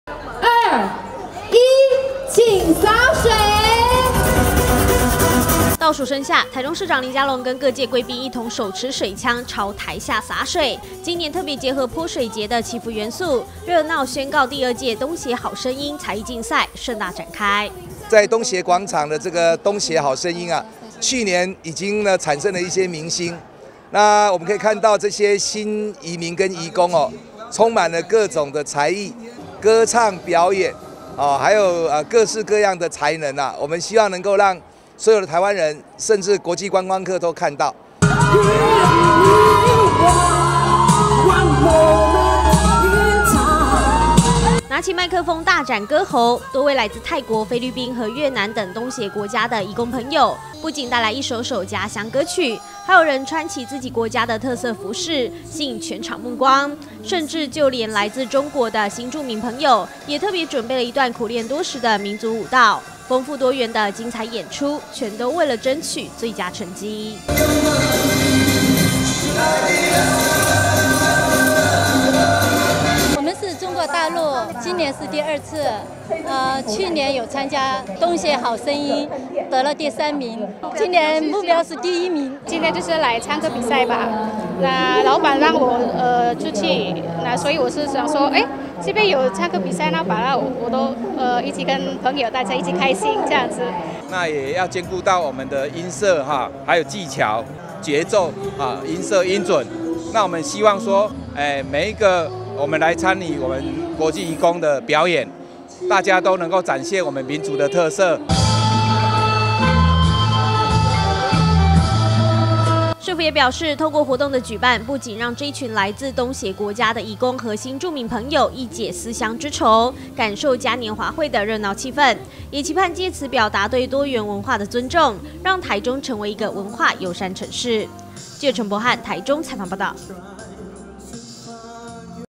大台中新聞-第二屆東協好聲音 移工.新住民大展才藝
打造台中多元文化城市，市府連續第二年在東協廣場，舉辦東協好聲音才藝競賽，吸引不少外籍移工和新住民熱情參與，不僅帶來苦練多時的各國歌曲和精采舞蹈，還有參賽者特別穿上自己國家的特色服飾，相當吸睛，而開幕典禮上，市府也特別結合潑水節的元素，由市長林佳龍與來賓一同向台下灑水，現場宛如嘉年華會，熱鬧滾滾。